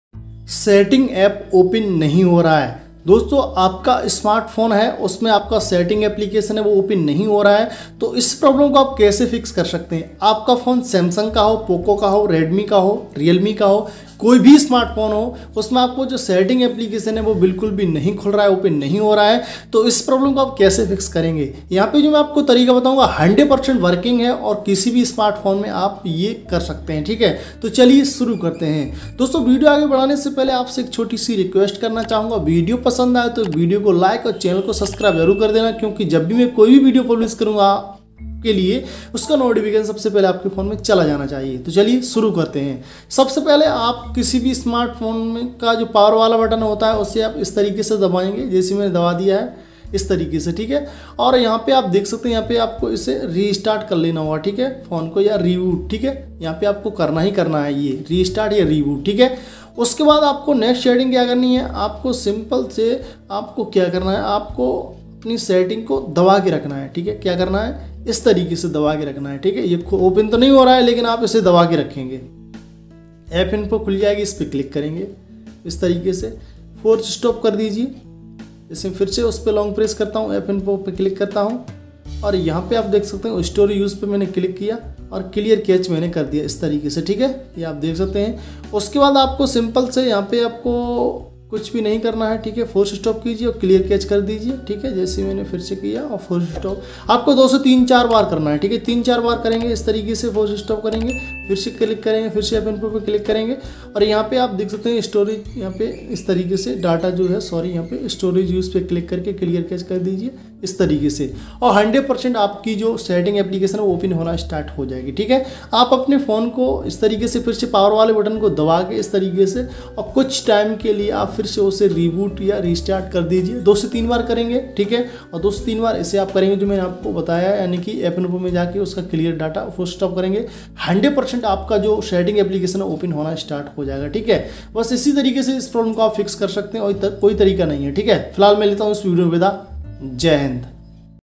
setting app open nahi ho raha hai | mobile ki setting open nahi ho raha hai
Dosto Aaj Ki Is Video Me Maine Aapko Bataya Hai - Mobile Ki Setting Open Nahi Ho Rahi Hai Toh Kaise Sahi Karen 100%

Video Queries -

setting app open nahi ho raha hai
mobile ki setting open nahi ho raha hai
mobile ki setting

Important Videos...